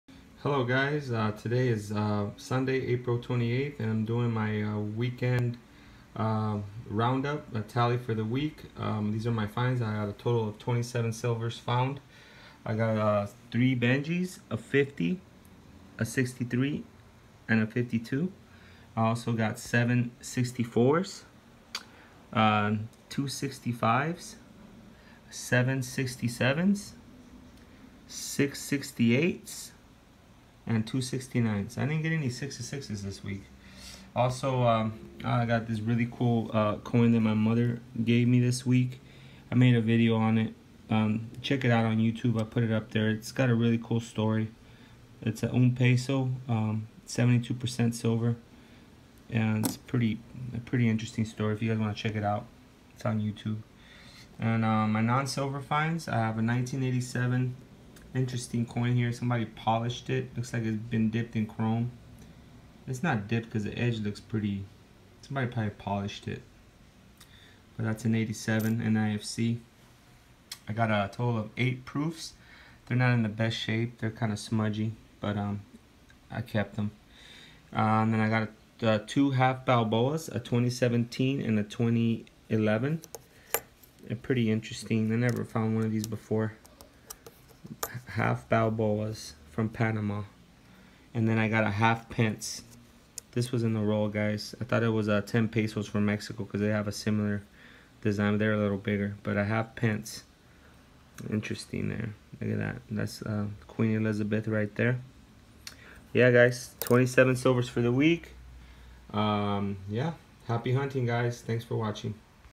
Coin roll weekly tally April 28th 2019
27 silvers found  10 90’s and 17 40’s